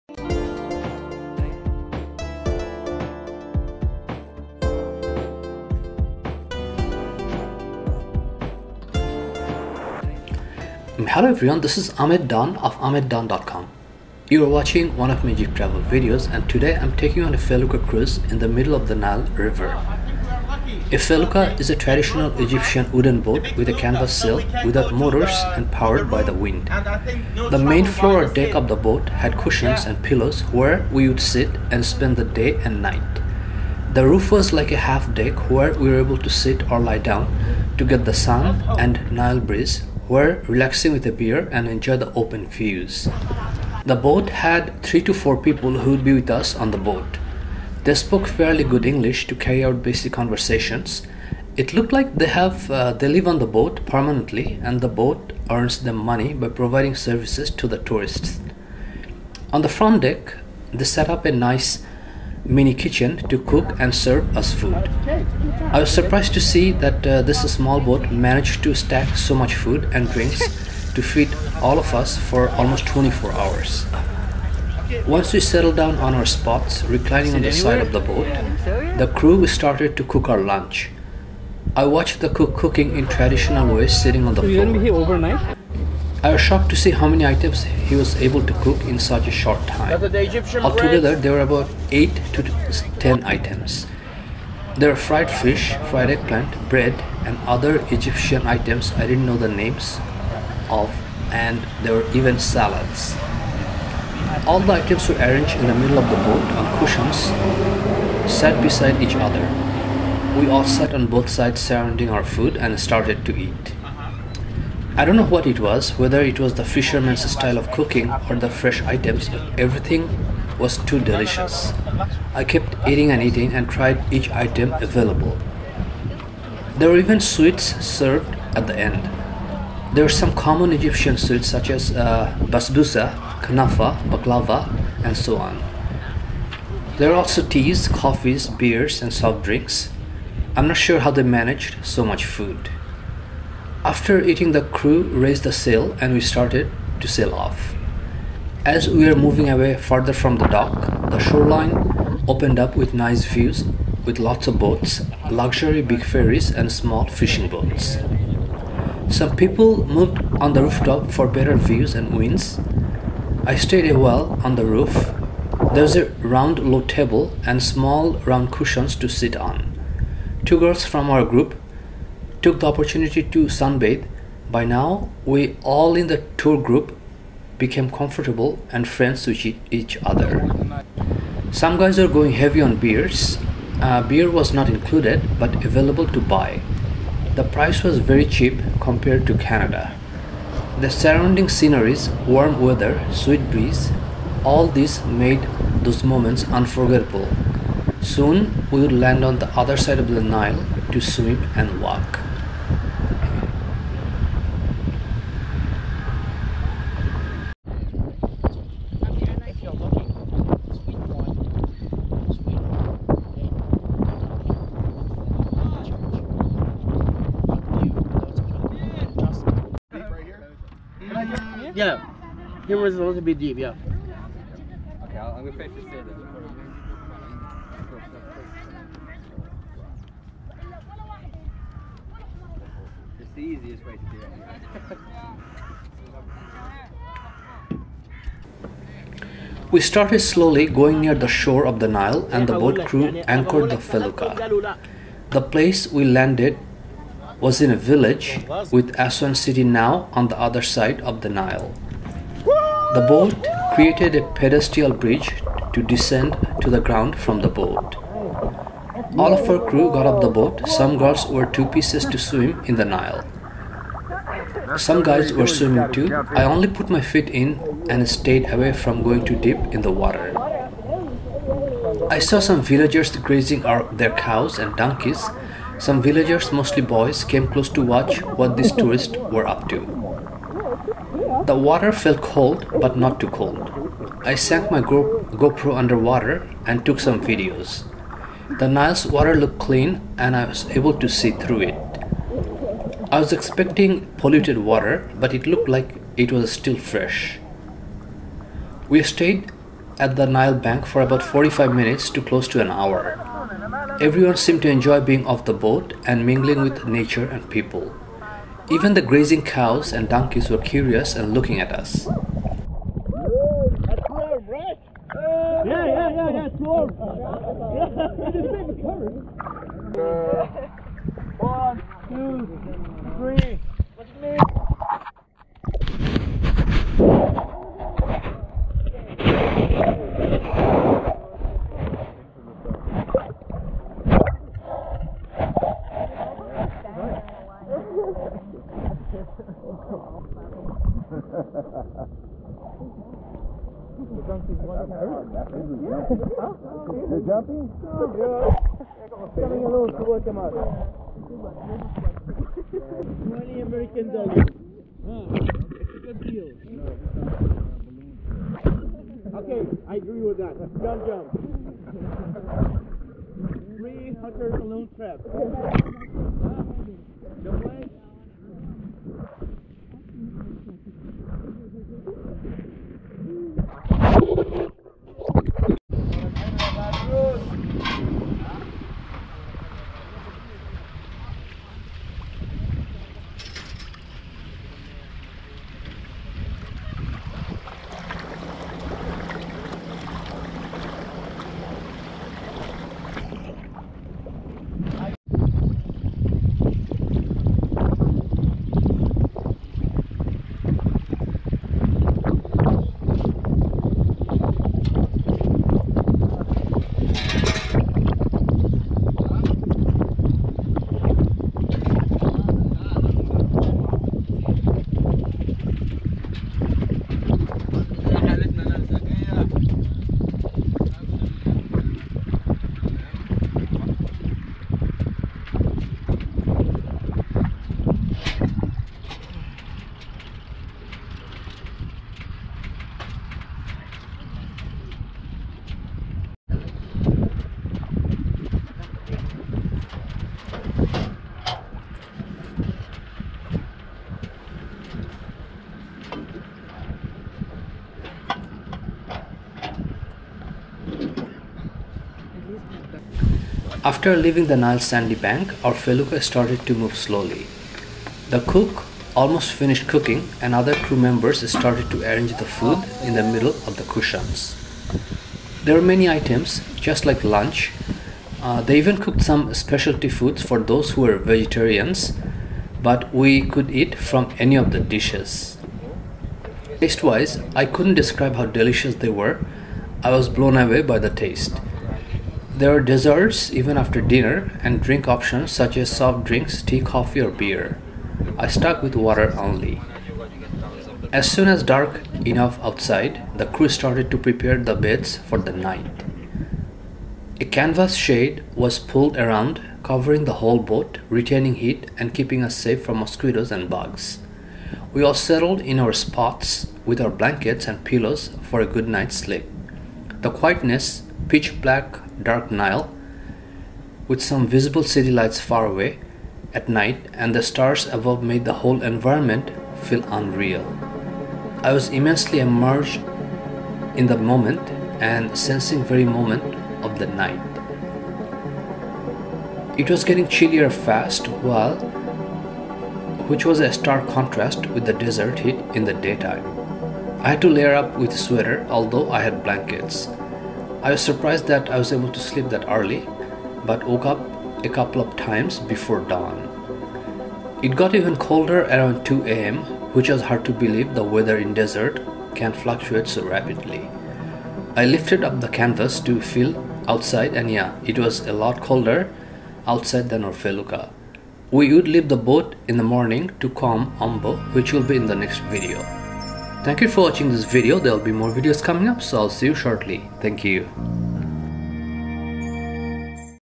Night in The Middle of the Nile In A Felucca Cruise Boat | Egypt Travel Vlog | 03D19
Nile River Cruise | Felucca Cruise Boat Tour | Egypt Travel | Sailing Boat Cruise on The River Nile in Egypt

Felucca is a sailing boat with no engine and runs by only sail and wind. The main floor of the Felucca had cushions and pillows. The roof had a place to sit and drink.

We also docked on the Nile shore to swim and relax. The crew cooked in the front part of the boat. It surprised me how they stacked so much food in that boat.

Next day, we left early morning. The crew showed us incredible hospitality, and we were sad to leave.

Today, I will take you with me to spend the night in the middle of the Nile river under the stars.

Chapters:
0:00 Intro
0:15 Ferry Dock, Aswan, Egypt
1:00 Inside the Felucca Cruise Boat
1:25 Lunch on the Felucca Cruise Boat
2:19 Felucca Cruise Boat Rooftop
3:45 On the Shore of the Nile River
7:33 Cooking on The Felucca Cruise Boat
8:35 The End of Our Felucca Cruise